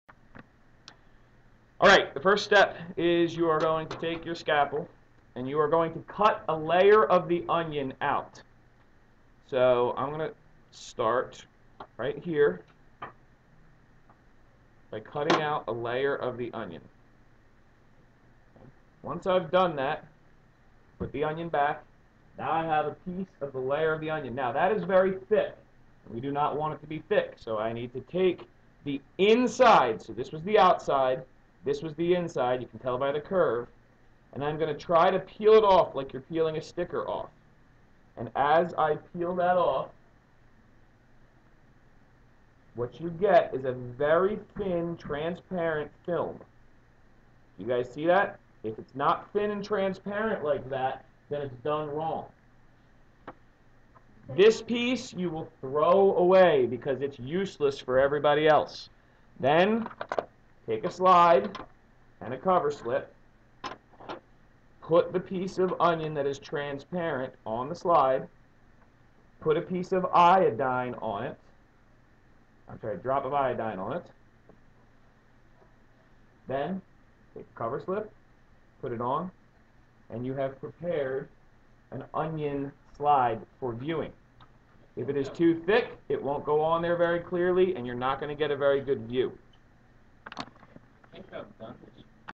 onion prep 2
Preparing a wet mount slide of onion skin for viewing under a microscope